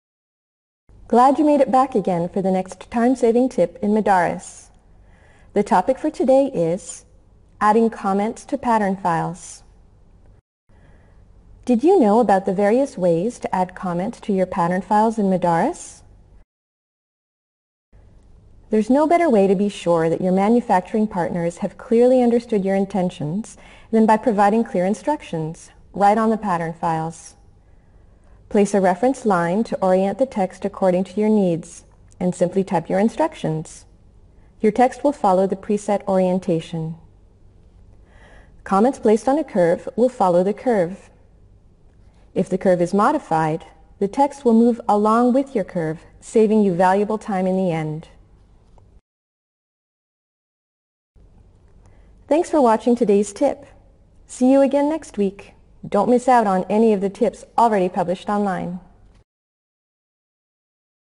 Lectra Modaris - Did you know # 19 - Adding comments to pattern files
Did you know about the various ways to add comments to your pattern files in Modaris? Watch the video to learn more!